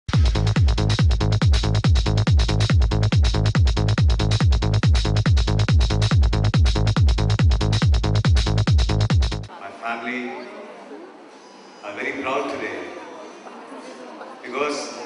Bollywood Movie Star Sanjay Dutt at Lavasa Women's Drive 2010
The Lavasa Women's Drive 2010 organized on Saturday March 7, which was attended by several Bollywood movie stars like Sanjay Dutt, Sharman Joshi, Rakesh Roshan at Bandra Reclamation Grounds.